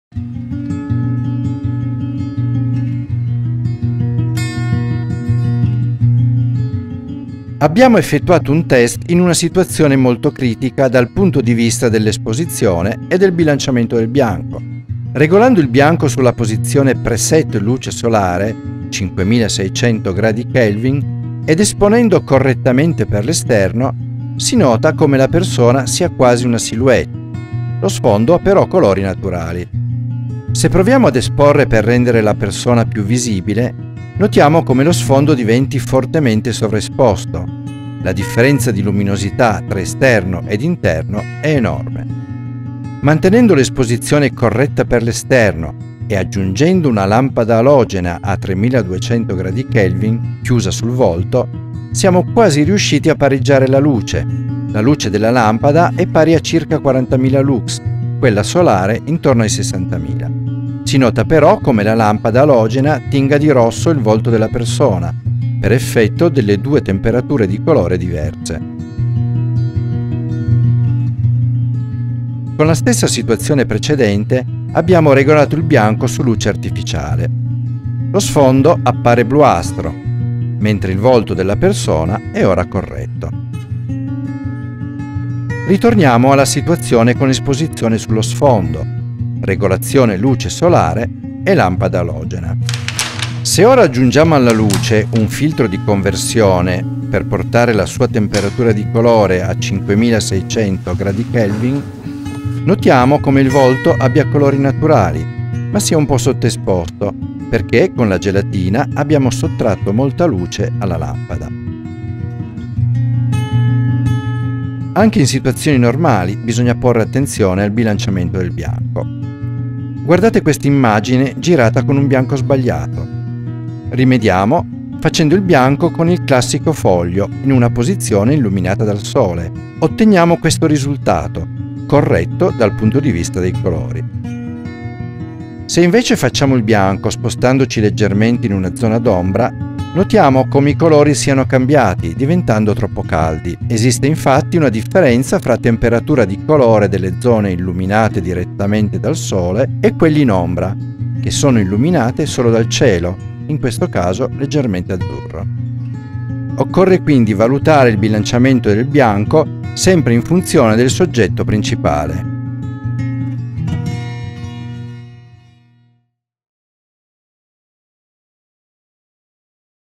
Videocorso - la ripresa video: esposizione e temperatura di colore.
Analisi di una situazione critica di  ripresa video-fotografica in forte controluce. Esposizione e calibrazione del bianco .